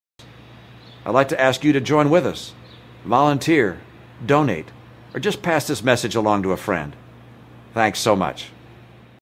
Mitt Romney with a Tiny Face
Mitt Romney with a Small Face. It's just so funny you can't stop laughing!